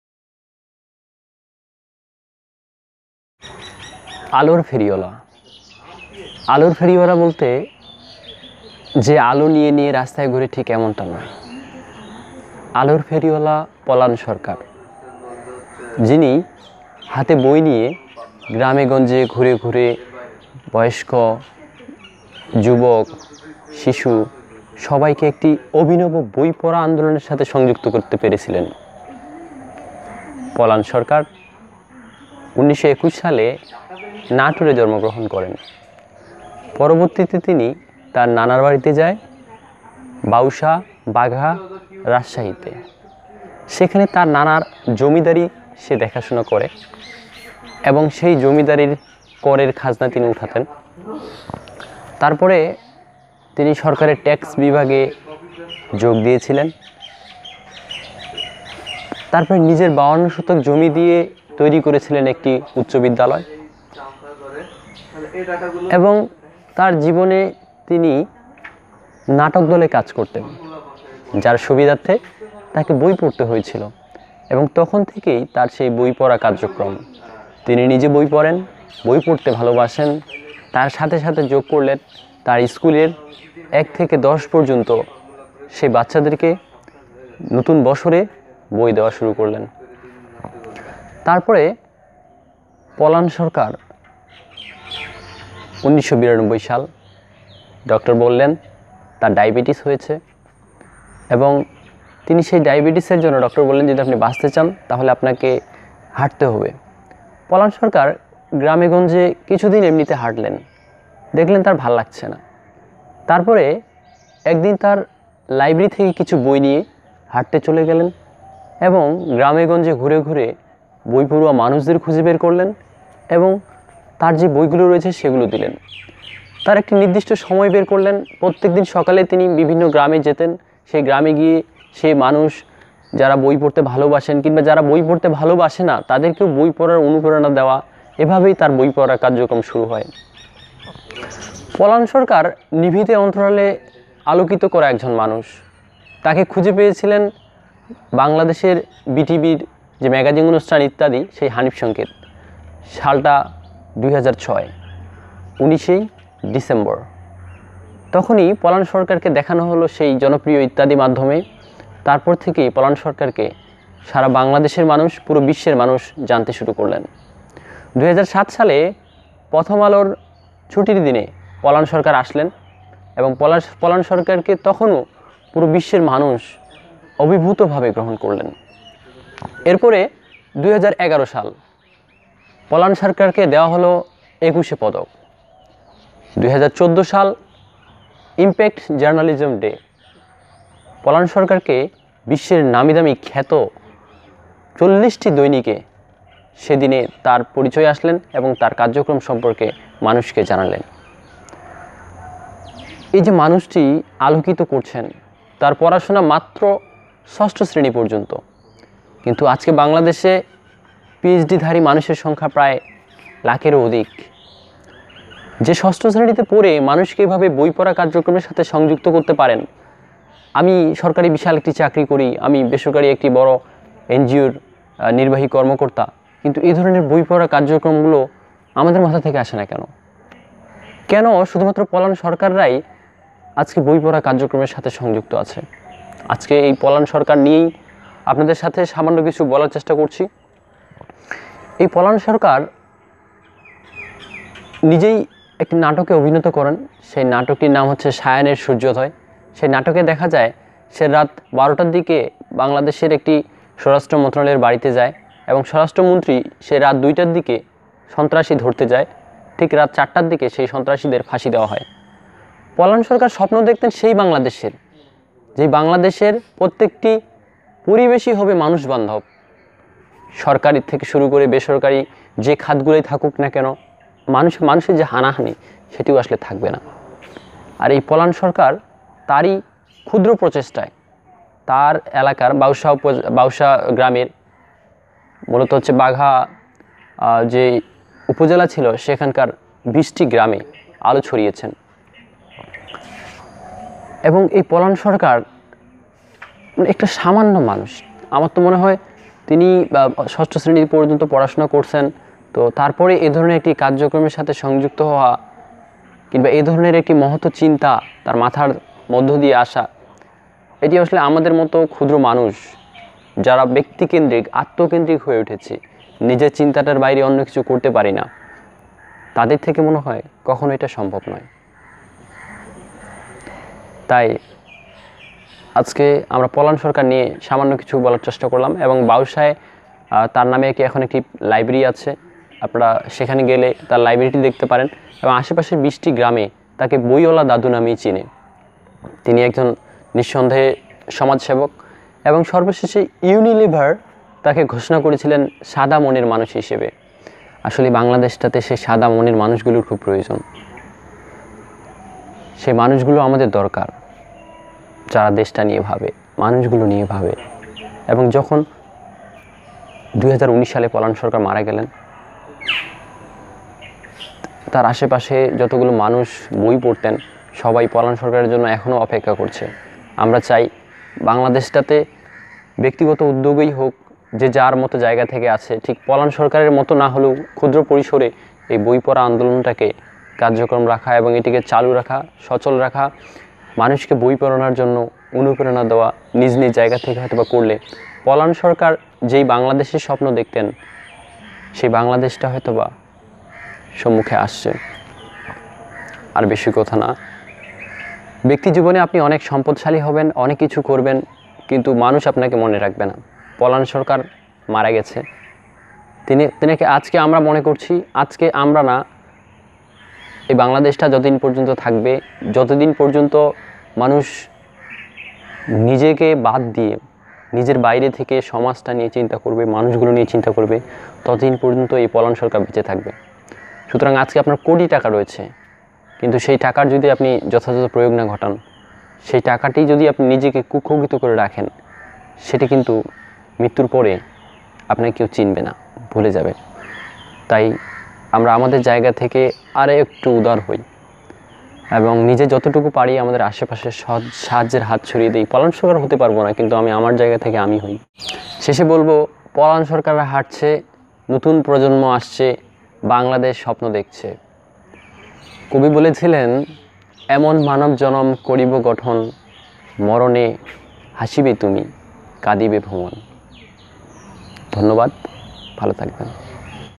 Short Biography of Polan Sarkar|আলোর ফেরিওয়ালা পলান সরকারের সংক্ষিপ্ত পরিচিতি|Research of Freedom
Short Biography of Polan Sarkar|আলোর ফেরিওয়ালা পলান সরকারের সংক্ষিপ্ত পরিচিতি|বইপ্রেমীর গল্প|Research of Freedom

পলান সরকার (৯ সেপ্টেম্বর, ১৯২১ - ১ মার্চ, ২০১৯) একজন বাংলাদেশি সমাজকর্মী। ২০১১ সালে সামাজিকভাবে অবদান রাখার জন্য বাংলাদেশ সরকারতাকে একুশে পদক প্রদান করে।পলান সরকার রাজশাহী জেলার ২০ টি গ্রামজুড়ে গড়ে তুলেছেন অভিনব শিক্ষা আন্দোলন। নিজের টাকায় বই কিনে তিনি পড়তে দেন পিছিয়ে পড়া গ্রামের মানুষকে। প্রতিদিন ভোরে ঘুম থেকে উঠে কাঁধে ঝোলাভর্তি বই নিয়ে বেরিয়ে পড়েন। মাইলের পর মাইল হেঁটে একেকদিন একেক গ্রামে যান। বাড়ি বাড়ি কড়া নেড়ে আগের সপ্তাহের বই ফেরত নিয়ে নতুন বই পড়তে দেন। এলাকাবাসীর কাছে তিনি পরিচিত ‘বইওয়ালা দাদুভাই’ হিসেবে।